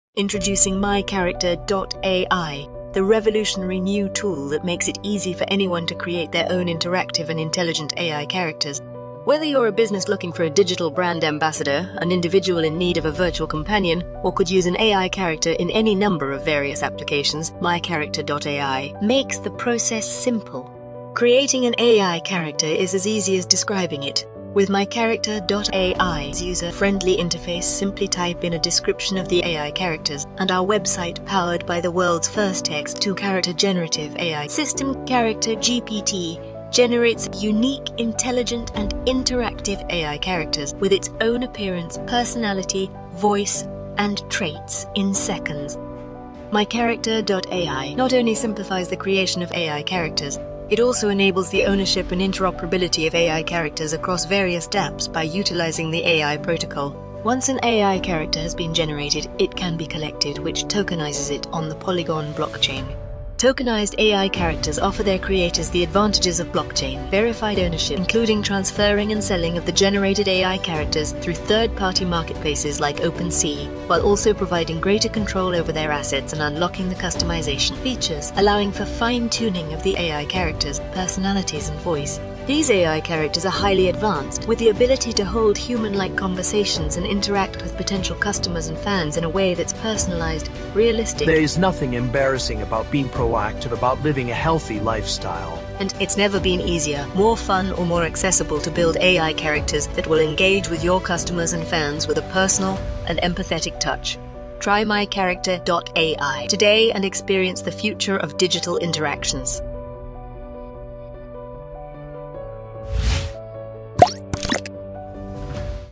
Introducing MyCharacter.ai - Enabling the Creation of AI Characters
A dApp built on the AI protocol that leverages the CharacterGPT Multimodal text-to-character AI system to generate realistic, intelligent, and interactive AI characters that are interoperable across all dApps on the AI Protocol, have the ability to be customized, and can be tokenized - enabling the ownership of your digital assets.

More info at MyCharacter.ai/about